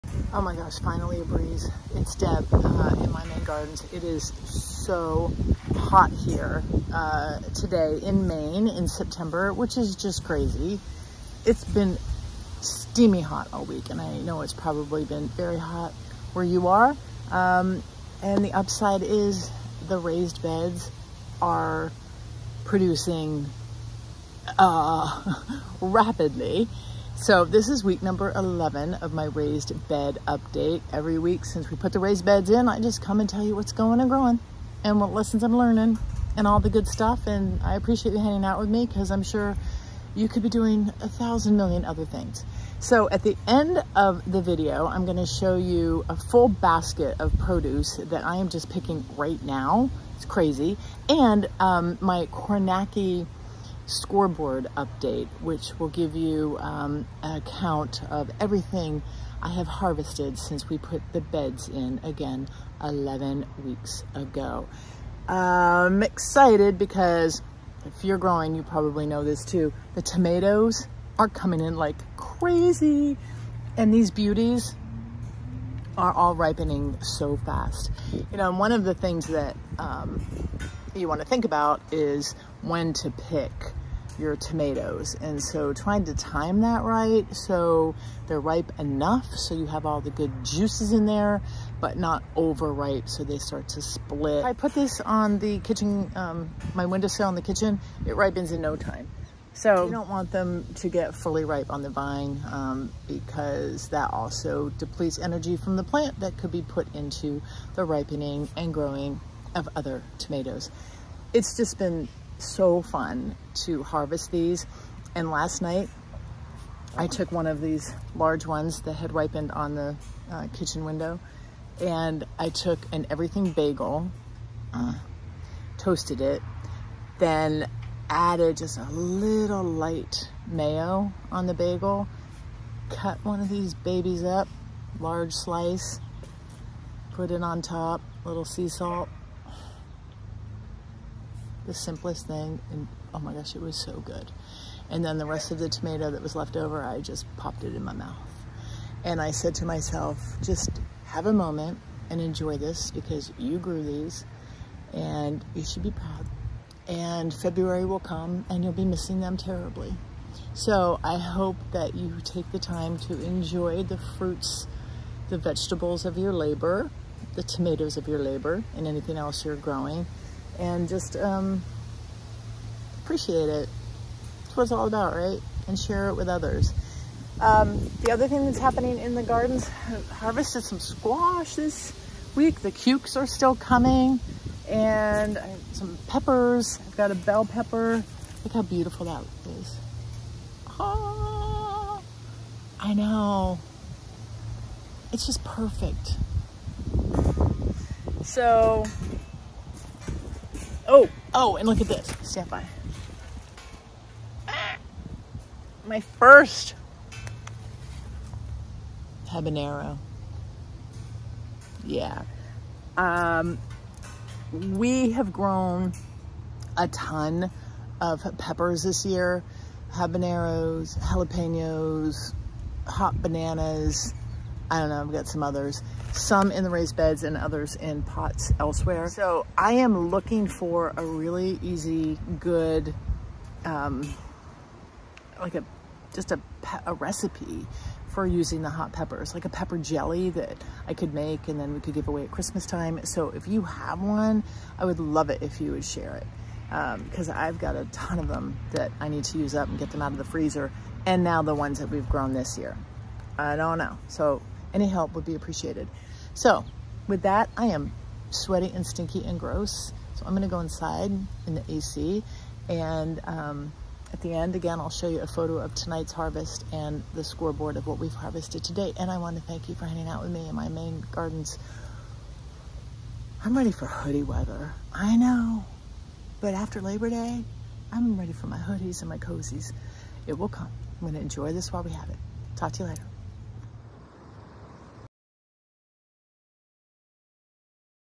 BIG harvest WEEK #11 raised beds update
My favorite tomato sandwich and what can I make with all these peppers?